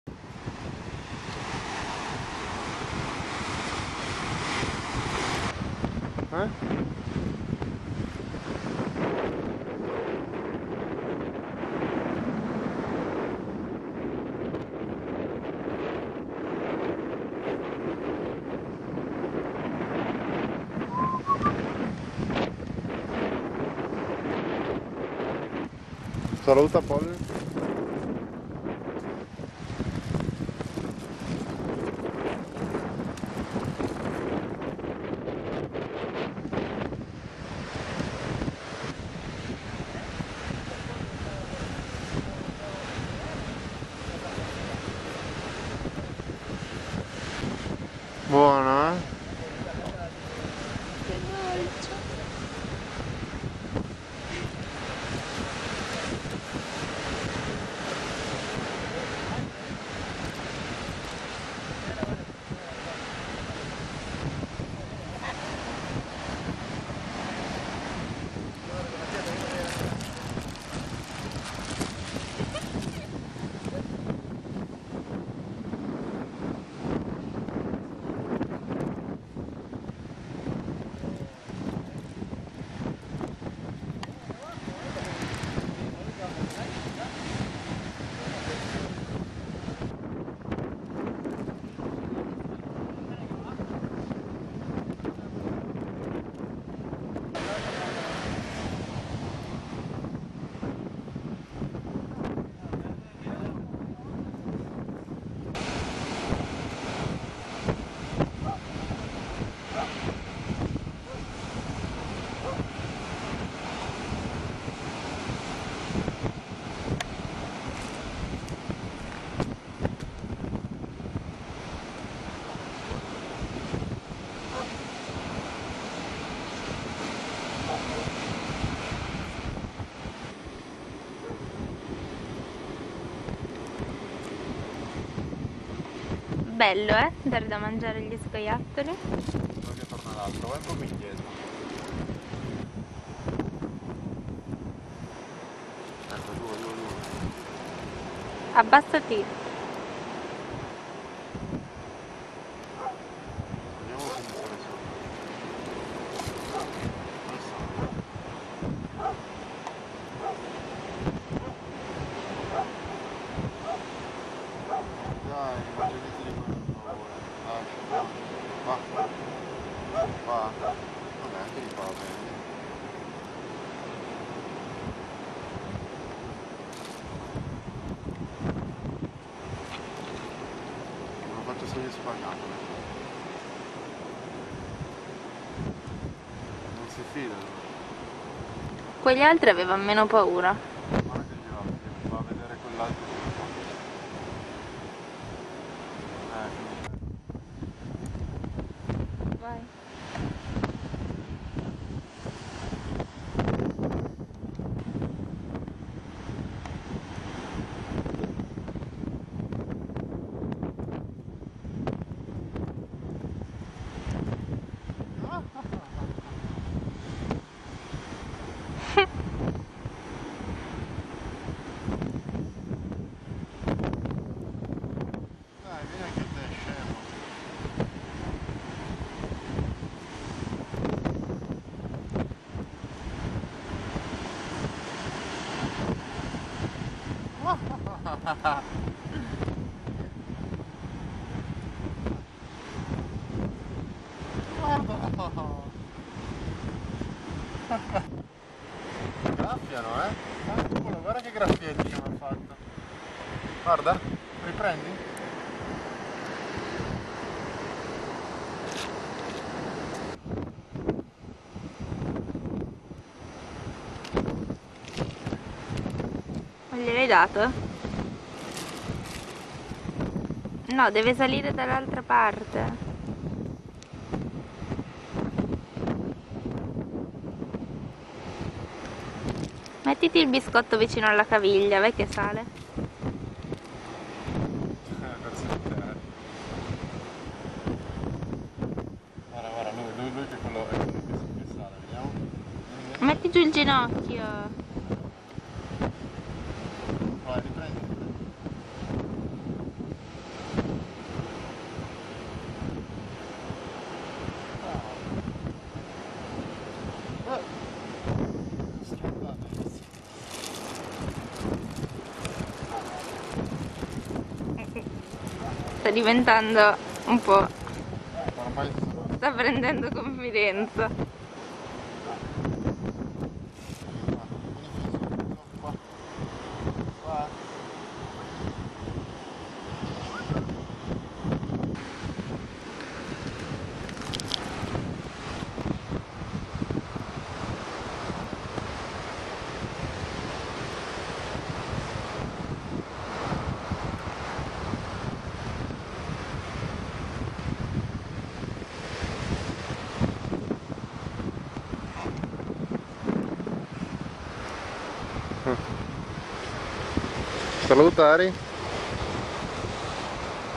Fabio e Arianna - Giocando con gli scoiattoli di Fuerteventura - Squirrels - Playa de Esquinzo
Io e Ari diamo da mangiare agli scoiattoli del deserto di Fuerteventura, vicino a El Cotillo. La spiaggia si chiama playa de esquinzo.